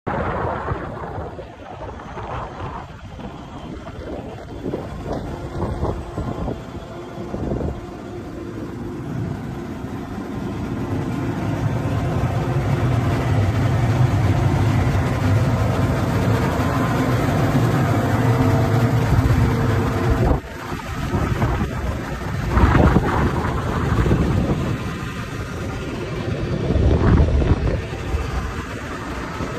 2022 JOHN DEERE S780 For Sale
2022 JOHN DEERE S780 with Rear Wheel Assit!  LIKE NEW, Serial # 1H0S780SHNT821132, 525 Engine Hours/ 369 Separating Hours, Premium Cab, John Deere PowerTech PSS engine 13.5 L with 473 HP, and PRWD!  Power Fold 400 bushel Grain Tank, Aactive Yield + Moist/Yield Sensor, Leather Seat, Premium Lights, Premium Radio, Gen4 4600 Monitor Command Center, and PRO Drive with Trans HarvestSmart!  Tracks ready axle, Contour Master, 580/85R42 Duals, Chopper, 28.5' Unloading Auger, Manual Adjust Vane Tailboard.  Located in Rugby, ND.